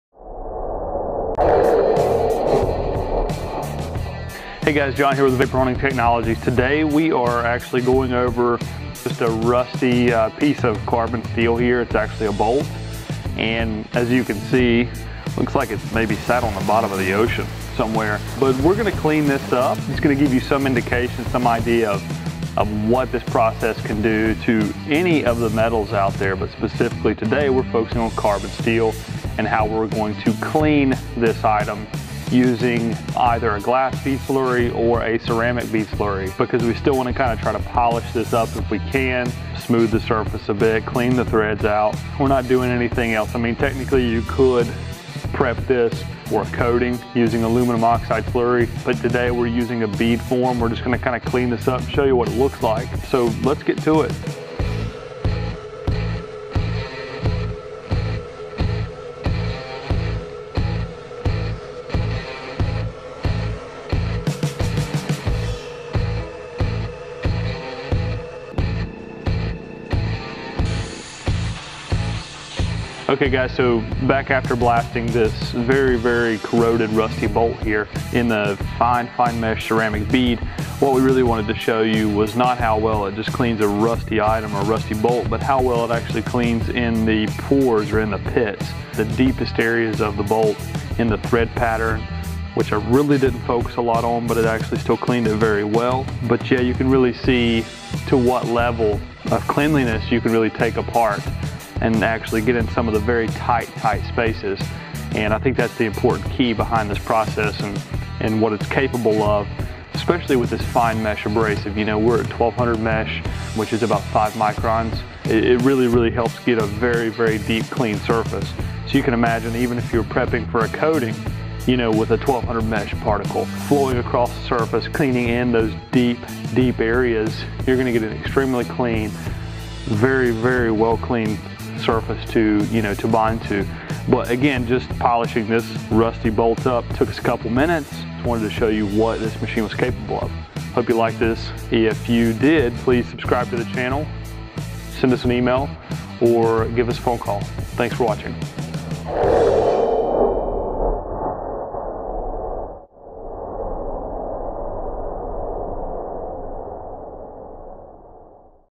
Ceramic Beads VS. Rusty Bolt, Ceramic Abrasive, Wet Blasting Ceramic Bead
We show you what Ceramic Beads can do to an old rusty bolt that looks like it lived in the bottom of the ocean for years but cleaned up in just minutes in our Wet Blaster.